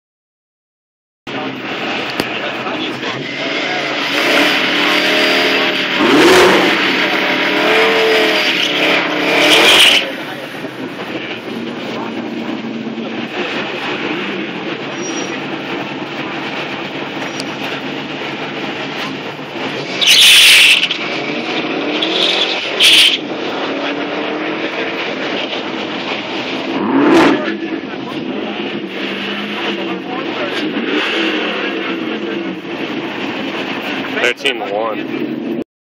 Mustang 13.16@105.12
1993 Mustang Coupe Stock Engine from Intake Manifold to Oil Pan, Stock H Pipe, Paxton SN2000 8psi. 90+ degrees
Ran a best of 12.98@107.29 this night.